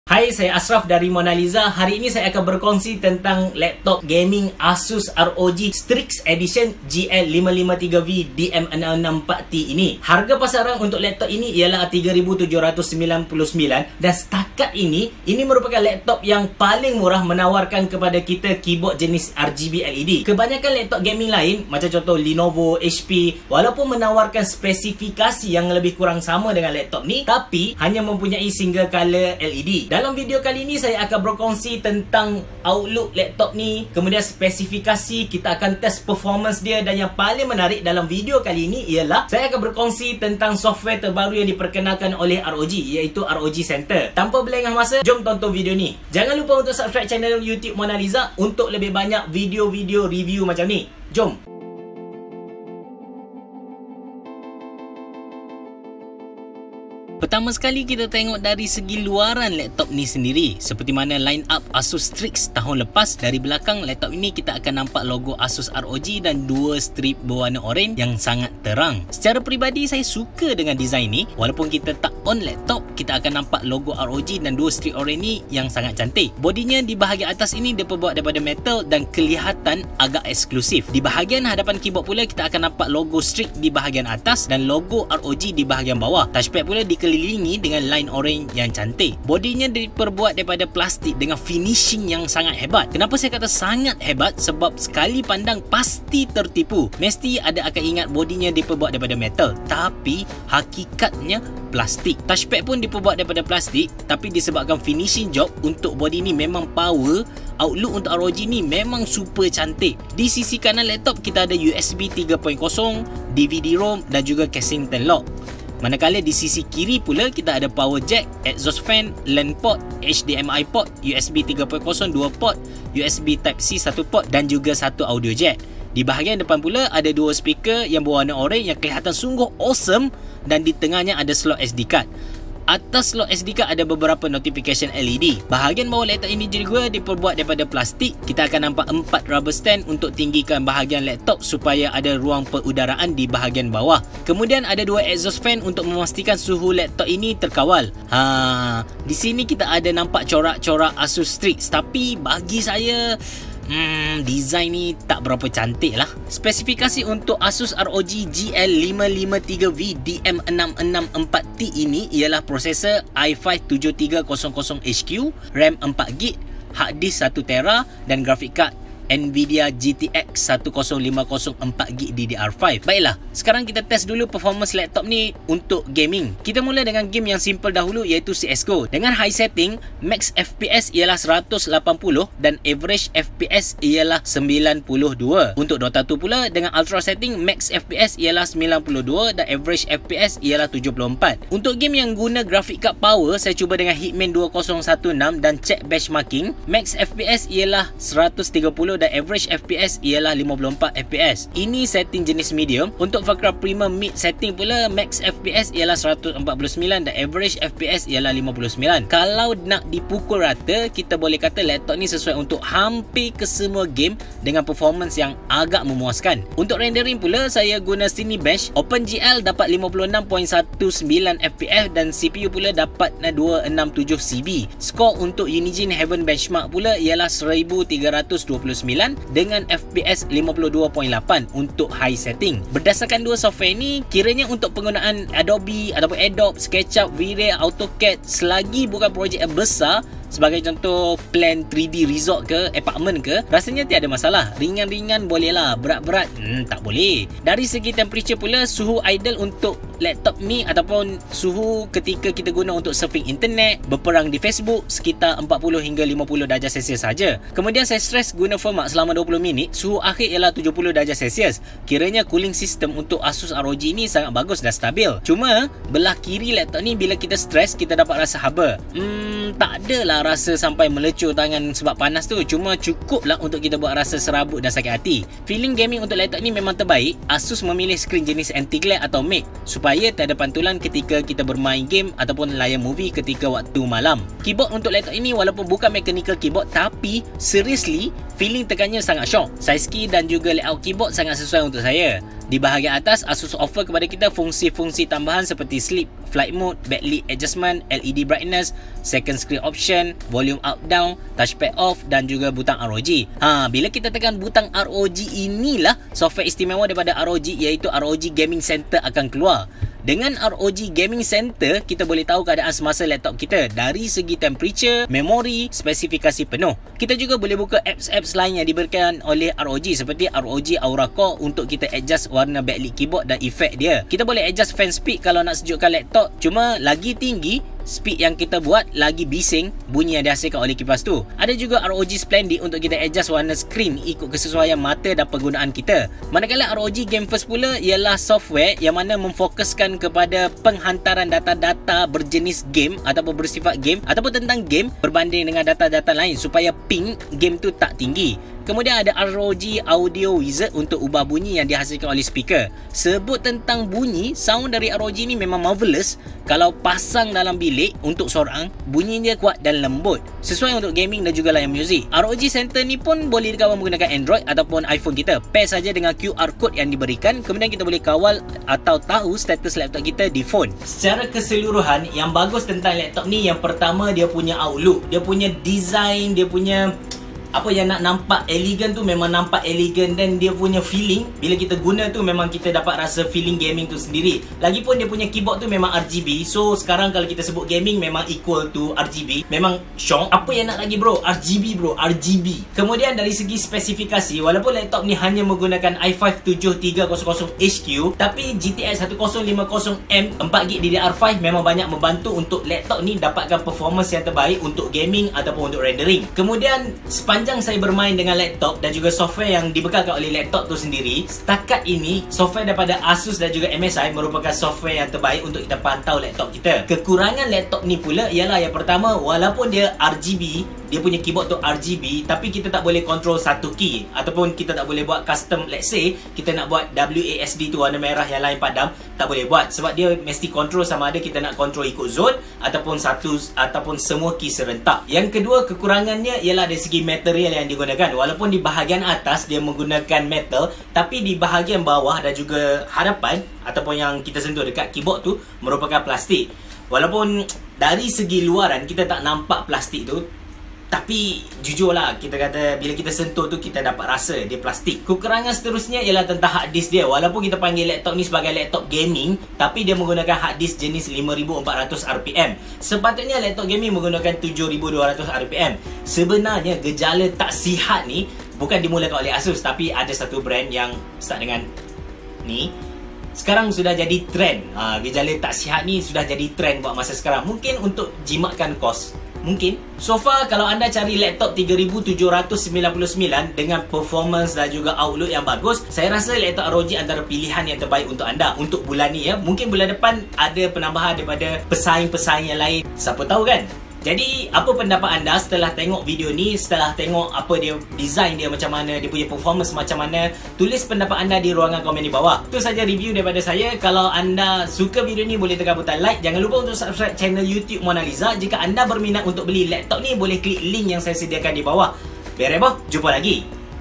Merecik Hawau! Review Asus ROG GL553VD DM664T
Nak Laptop Gaming bawah RM4,000 dengan Spesifikasi yang MANTAP dan juga Keyboard jenis RGB?

Asus ROG GL553VD-DM664T ialah jawapannya!

Spesifikasi :
i5-7300HQ - RAM 4GB - HDD 1TB - Nvidia GTX1050M 4GB DDR5 - 15.6"Inch Full HD IPS Panel

Tonton video review saya tentang Laptop Asus ROG GL553VD-DM664T ini untuk tahu dengan lebih lanjut tentang design, perfomance dan juga keistimewaan yang tiada pada Laptop Lain.